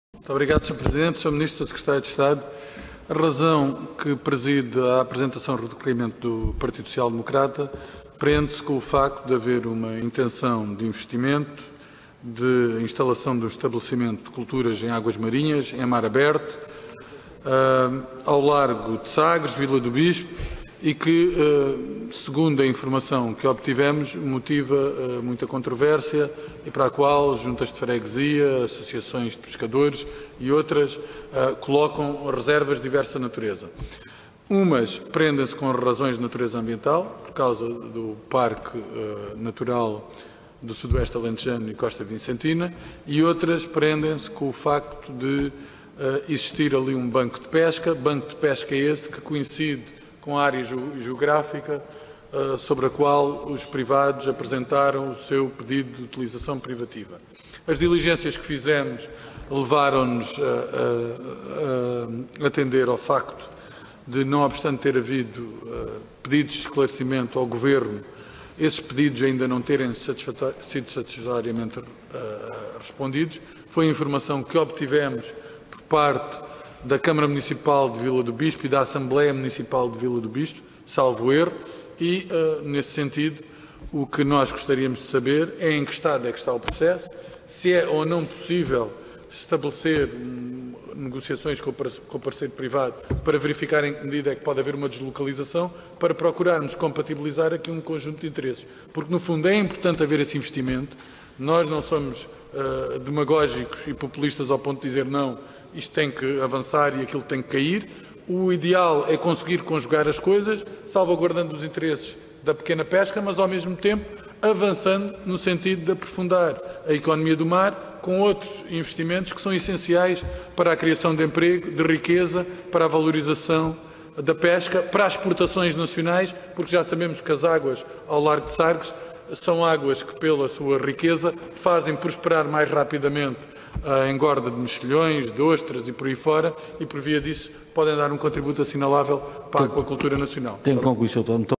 Cristóvão Norte questiona Ministro do Mar
Intervenção na Comissão de Agricultura e Mar.
7 julho 2020